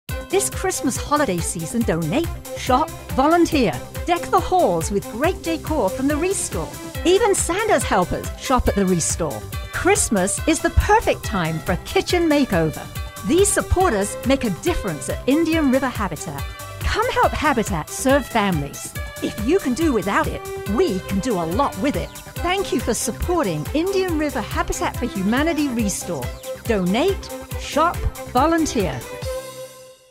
"Home for Christmas" for Indian River Habitat for Humanity Restore, BWP producer Robert Goodrich
Produced for Indian River Habitat for Humanity Restore by BluWave Productions West Palm Beach producer Robert Goodrich
Post-Production:  Brian Terrell
Comcast Spotlight Account Executive:  Tanya Costa